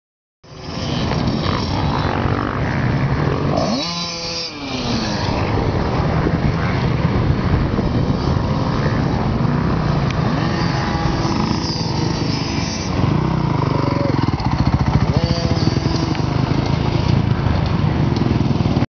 Jake's 1st moto
Jake's first laps on the VC pee wee track.